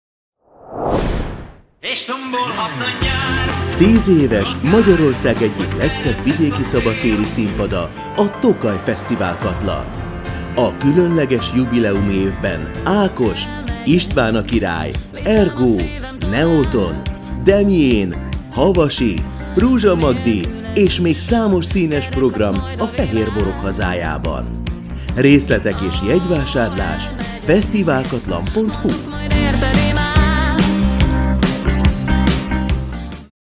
10 éves a Tokaj Fesztiválkatlan - A Tokaj Fesztiválkatlan 2024-es programjai
10 éves Magyarország egyik legszebb szabadtéri színpada, a Tokaj Fesztiválkatlan! A különleges jubileumi évben visszatérnek népszerű előadóink, de számos színes program várja az érdeklődőket a fehérborok hazájában!